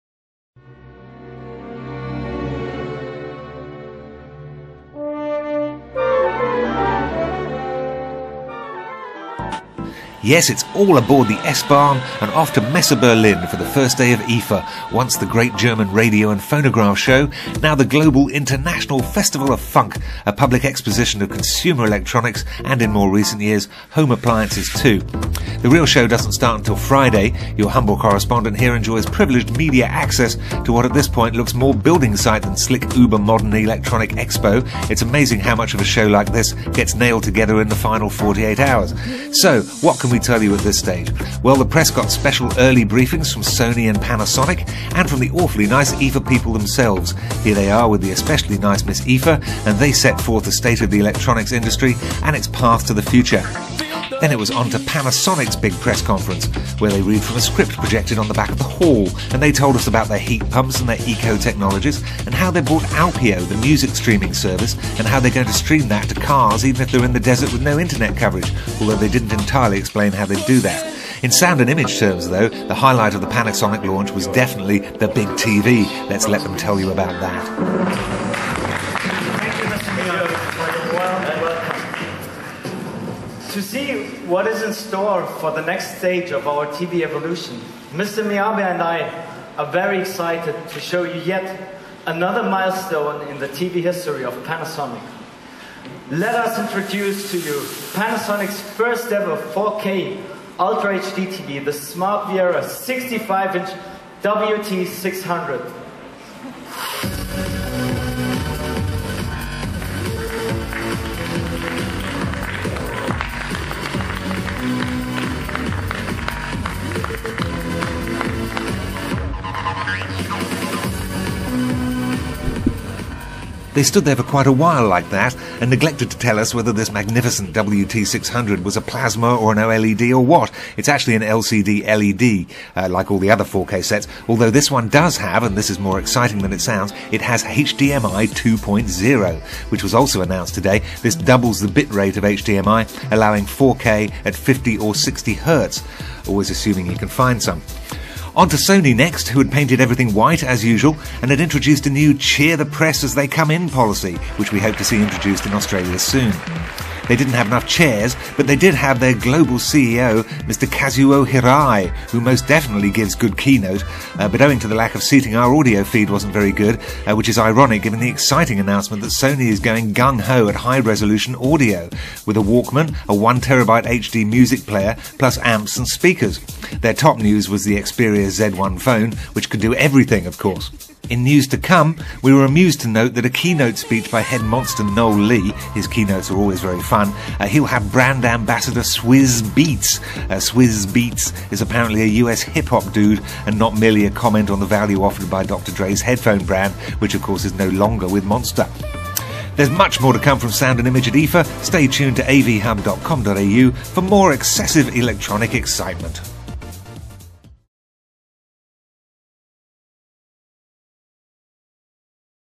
IFA Berlin 2013
Two days before the show begins we attend press showings by Panasonic including the launch of the 4K 50/60Hz WT600 television, and a big white Sony room where the Sony staff quite rightly applaud the press representatives as they come in. Plus HDMI 2.0 and a load of new hi-res audio gear. Enjoy.